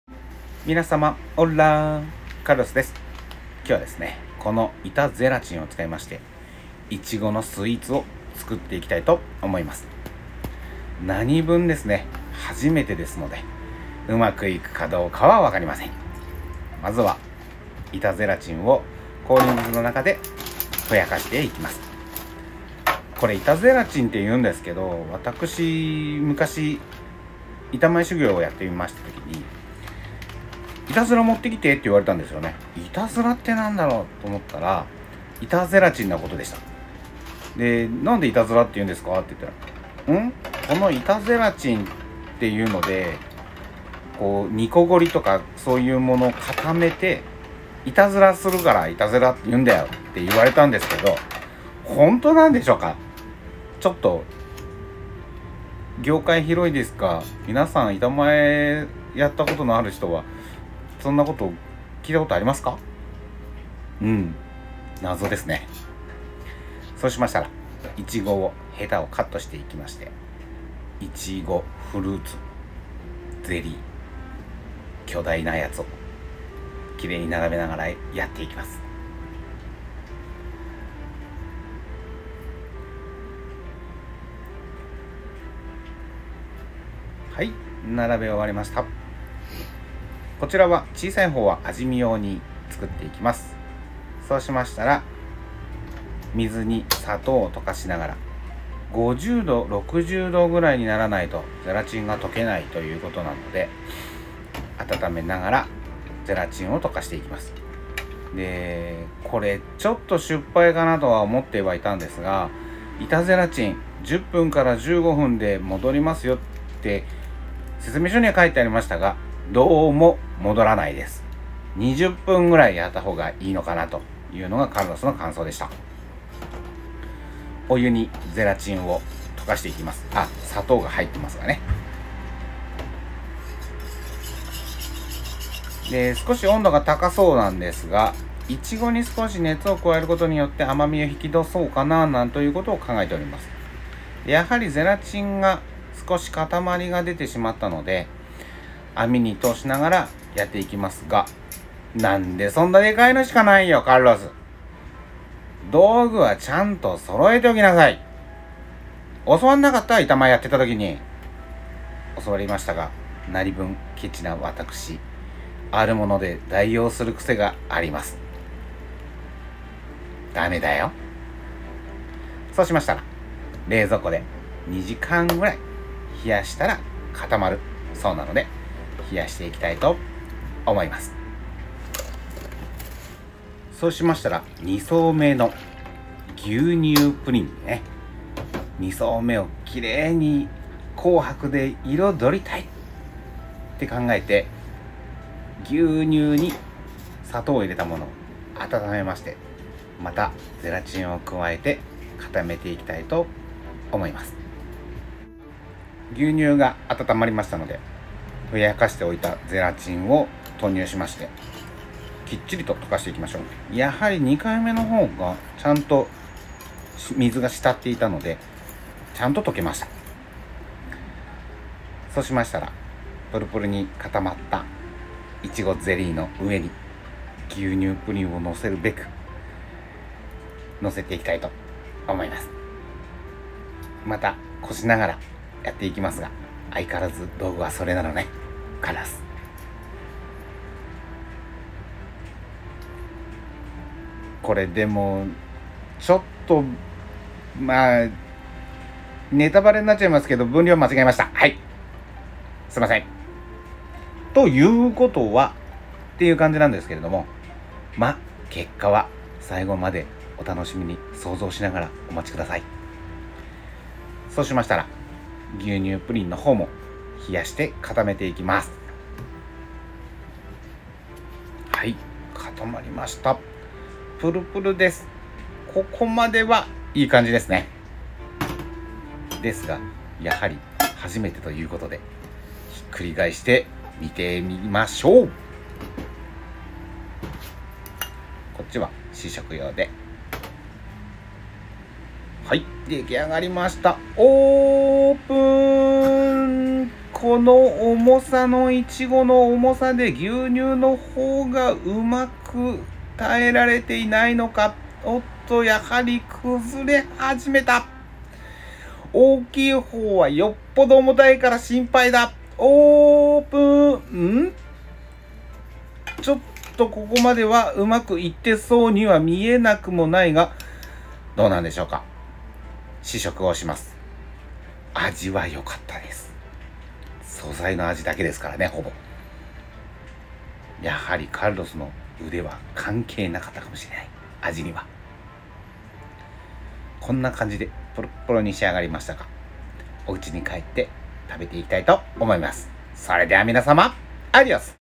【sweet】板ゼラチンでイタズラしてプルぷる！！
イチゴの美味しい季節です！板ゼラチンでイタズラしてプルぷるにしたかったのに…sweet